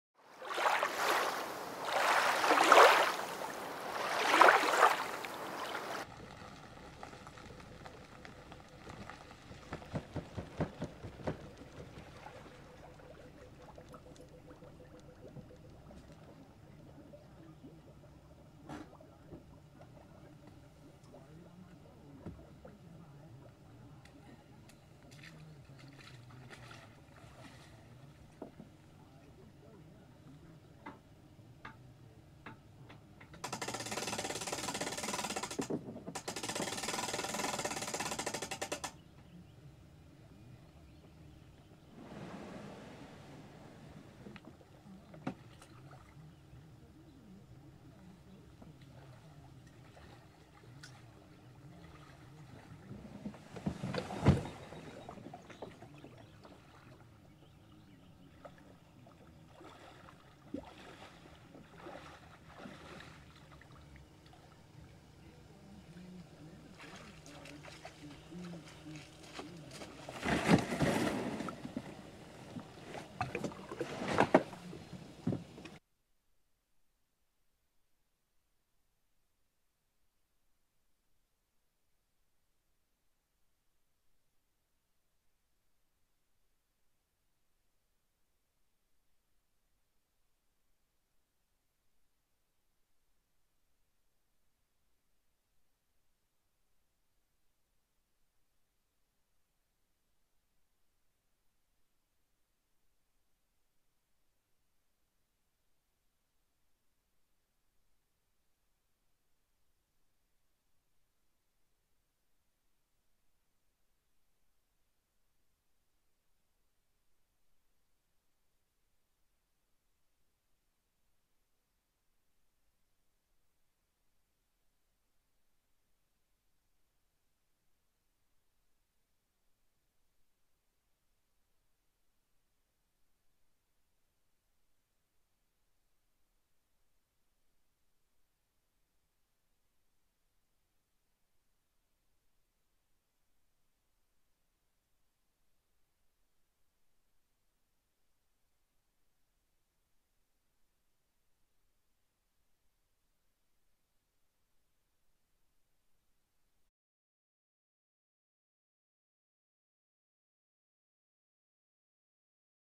Kayak Trailer Launch & Load || Hobie PA12 || Kayak Fishing
Just a quick video of me launching and reloading my kayak with the trailer.